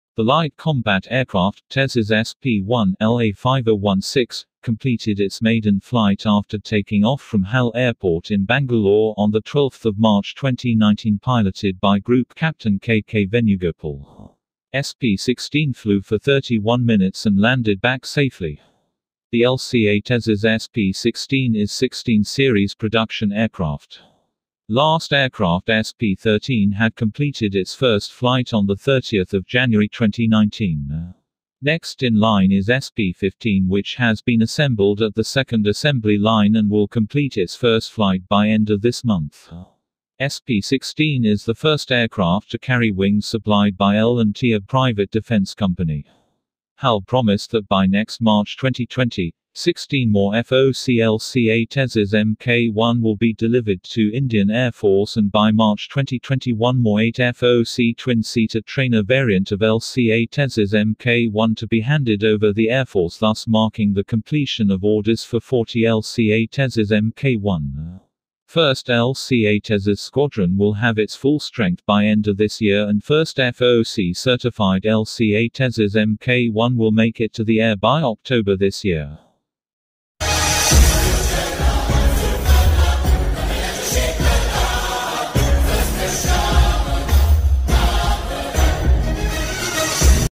LCA Tejas SP-16 (LA–5016) complete its first flight. |HAL LCA Tejas SP-16 first fligh.
The Light Combat Aircraft (LCA) – TejasSP16 (LA-5016 ) completed its maiden flight after taking off from HAL Airport in Bangalore on 12-March-2019 piloted by Gp Capt K K Venugopal (Retd). SP-16 flew for 31 mins and landed back safely.

The LCA Tejas SP-16 is Sixteen Series Production aircraft. Last aircraft SP-13 had completed its first flight on 30-January-2019. Next in line is SP-15 which has been assembled at the second assembly line and will complete its first flight by end of this month. SP-16 is the first aircraft to carry Wings supplied by L&T a Private defense company.

HAL promised that by next March 2020, 16 more FOCLCATejasMk1 will be delivered to Indian Air Force and by March 2021 more 8 FOC Twin Seater Trainer variant of LCA-Tejas Mk1 to be handed over the airforce thus marking the completion of orders for 40 LCA-Tejas Mk1.

First LCA-Tejas squadron will have its full strength by end of this year and first FOC certified LCA-Tejas Mk1 will make it to the air by October this year.

Recommended video:
Recommended video:

About Channel : ArmedForce channel on YouTube where you will find news update, defence technology and video related to the indian space and defence.